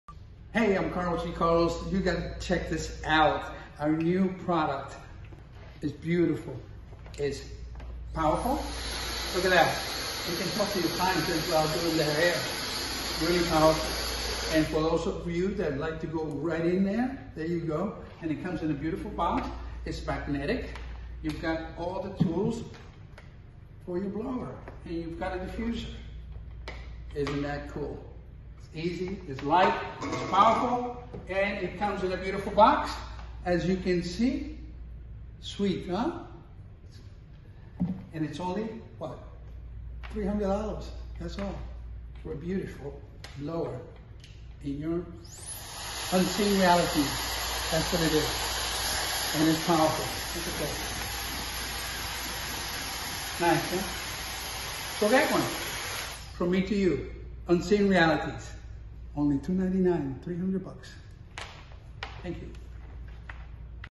UR Blow Dryers - Only $299
Features an innovative, patented minimalistic look and pioneering design with Intelligent heating design with NTC constant temperature control. Comes with LED indicators a digitalized brushless motor and special design with infrared function. Sophisticated and unique Ice color that dries the hair 300% faster.